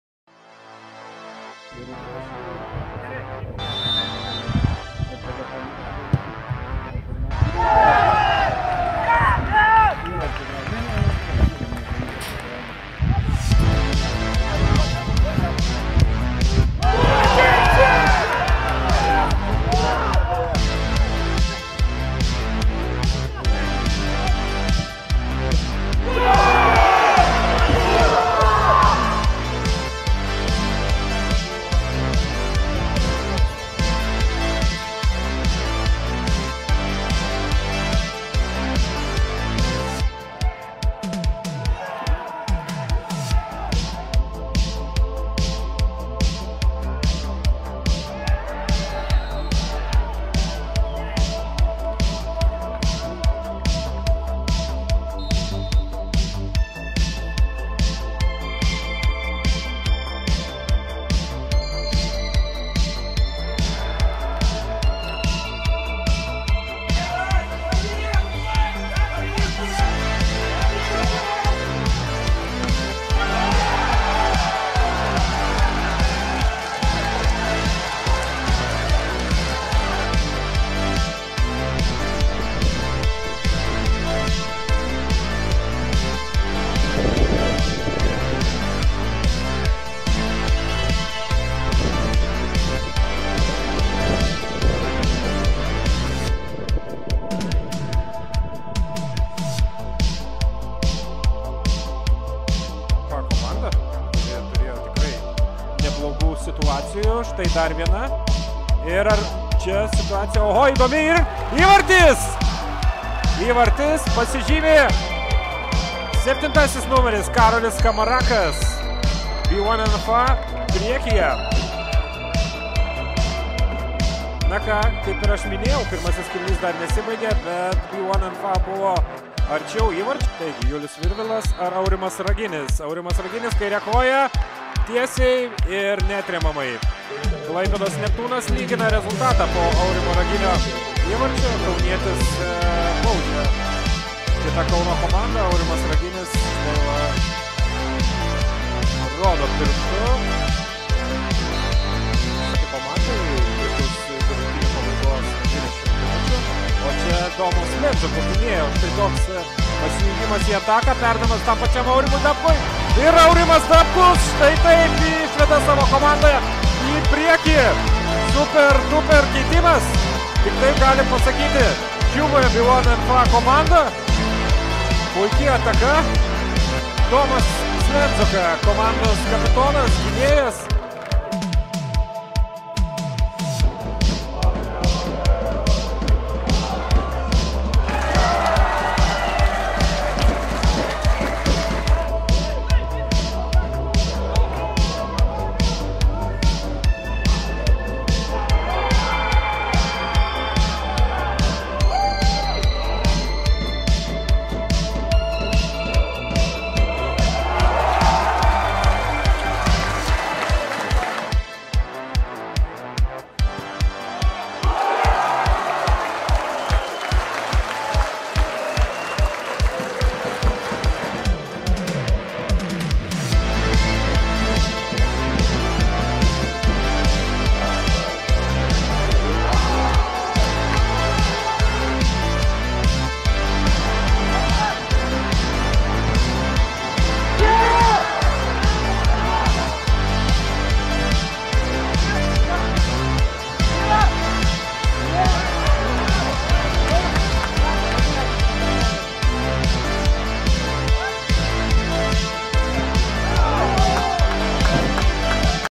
VIII turo įvarčiai
Šiauliai B 0:3 BFA
Ekranas 1:1 Minija
Nevėžis 1:0 Atmosfera
Marijampolė city 0:0 Banga B
Garliava 0:1 Riteriai B
Be1 NFA 2:1 Neptūnas
Dainava 3:2 Babrungas
Panevėžys B 0:3 Žalgiris B